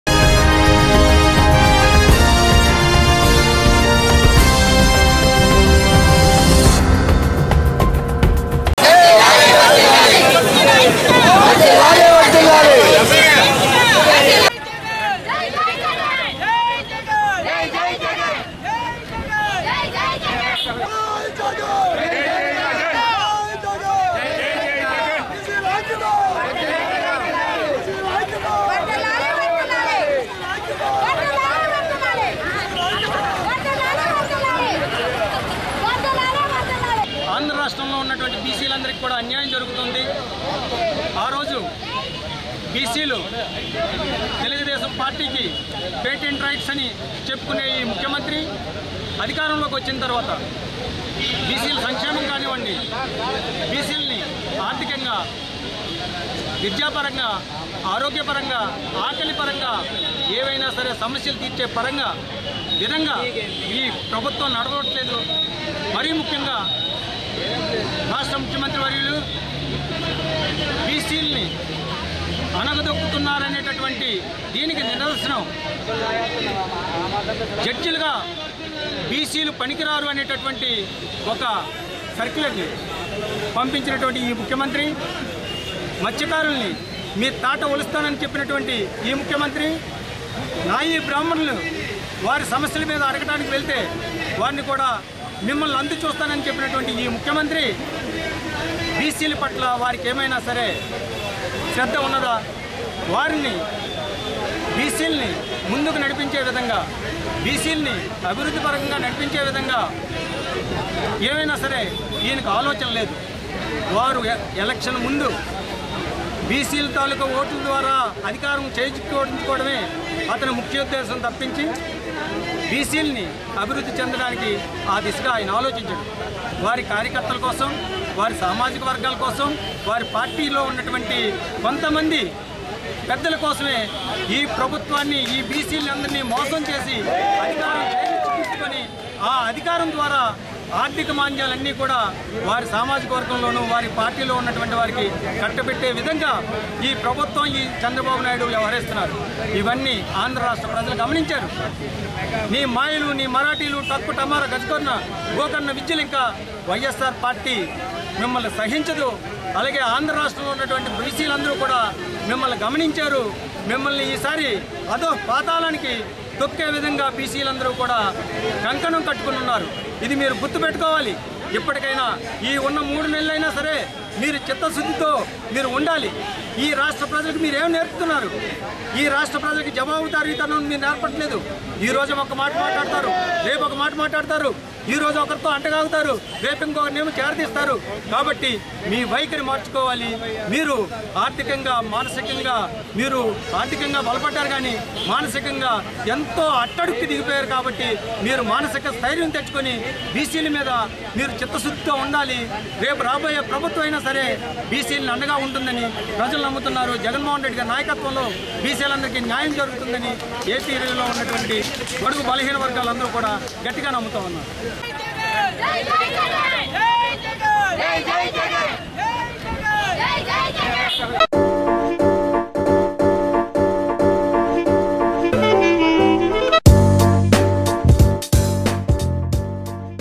ens balu#ysrcp vizag bc wing president patrudu fire on ap cm#చంద్రబాబుపై నిప్పులు చెరిగిన పాత్రుడు#
బీసీలను అణగదొక్కిన టీడీపీ సీఎం చంద్రబాబుకి నూకలు చెల్లాయంటూ నిప్పులు చెరిగిన వైఎస్సార్సీపీ బీసీ వింగ్ అధ్యక్షులు పాత్రుడు.